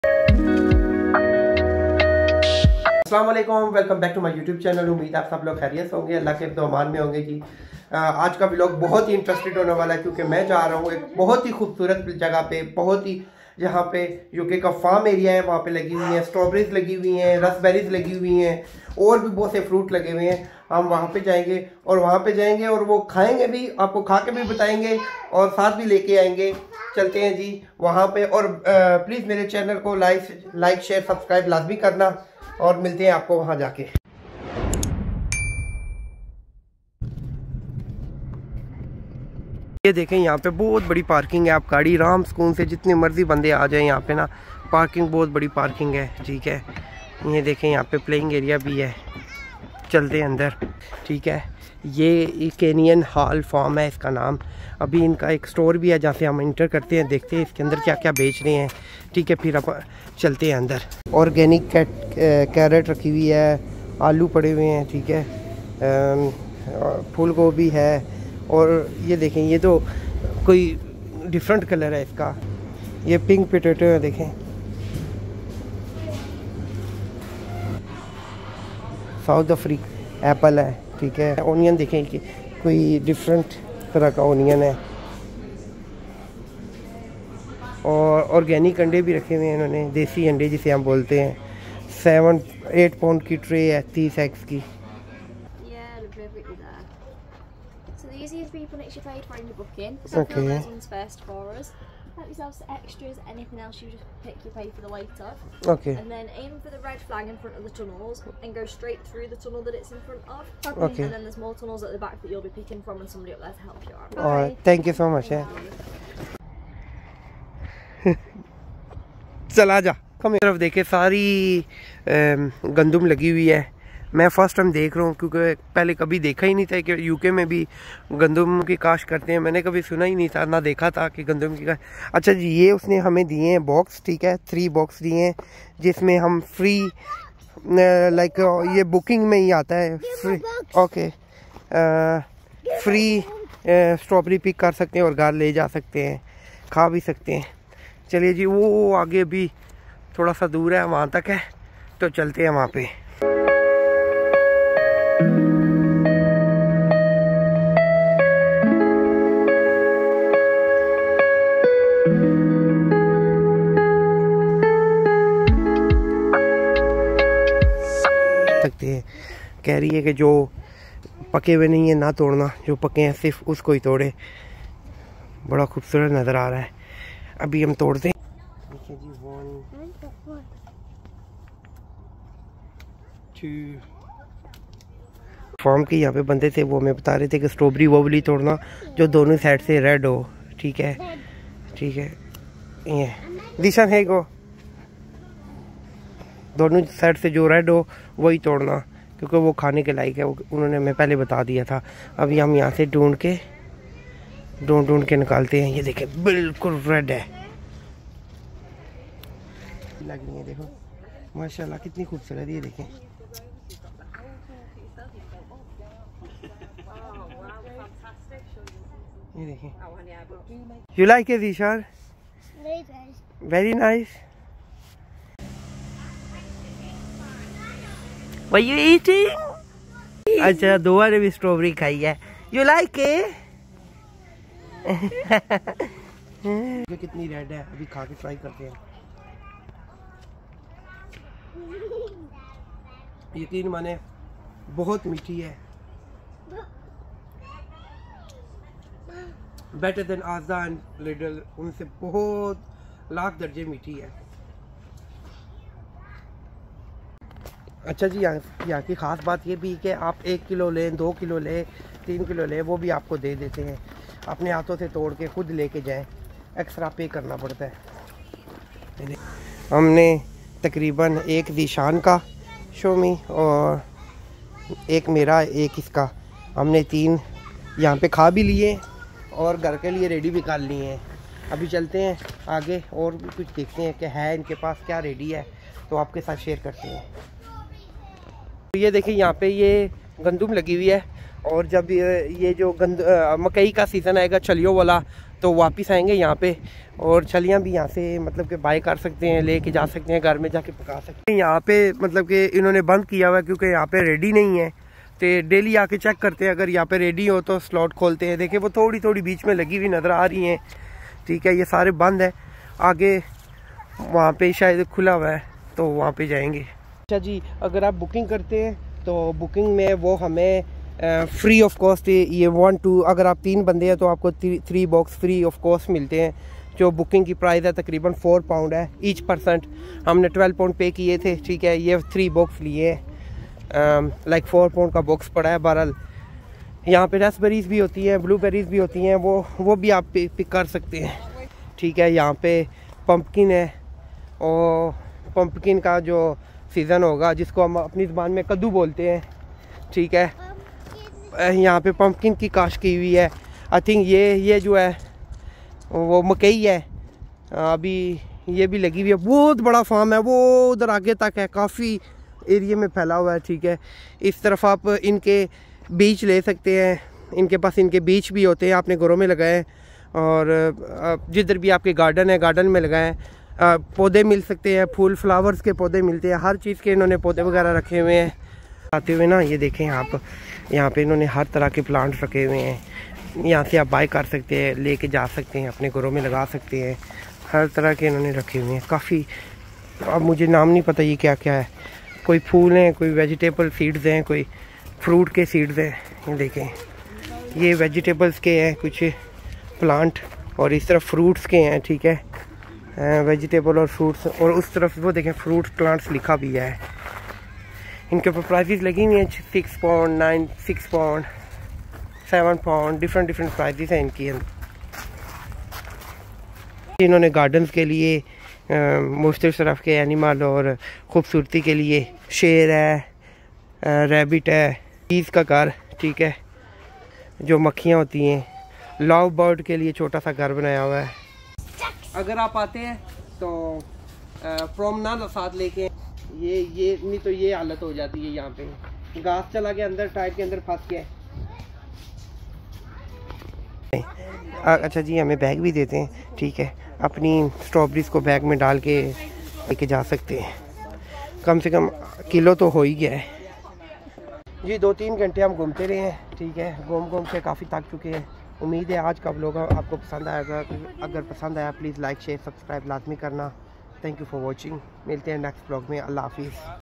Trip to Strawberry farms in England/ Kenyon Hall Farm
Trip to Strawberry farms in England/ family vlog

Umeed hain hap Sab haryat se hon ghe Allah hap Sab ko hefzo emaan mein rakhe.